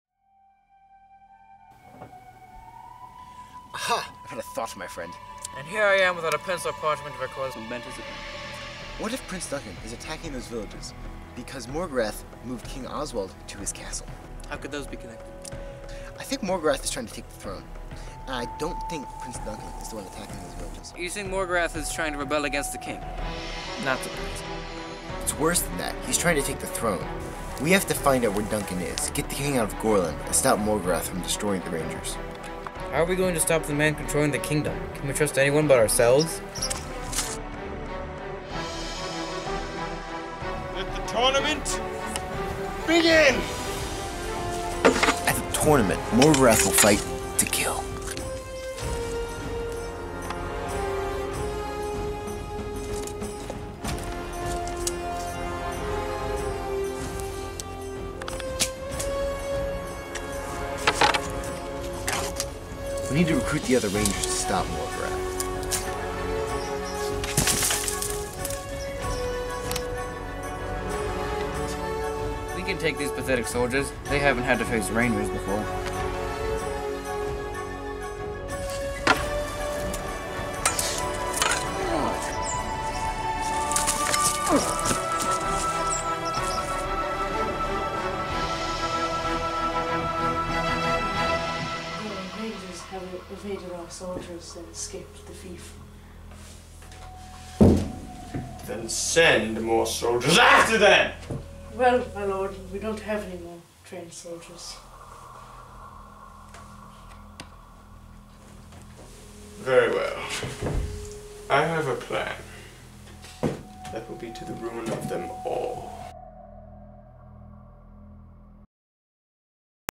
HCPL Film This! 2018 (Ranger's Apprentice: The Early Years: The Tournament at Gorlan)
The filmographic representation of the book trailer for said book title for the HCPL Film This! competition. Done by Nathan B., and Jack, Sam and Jonah M.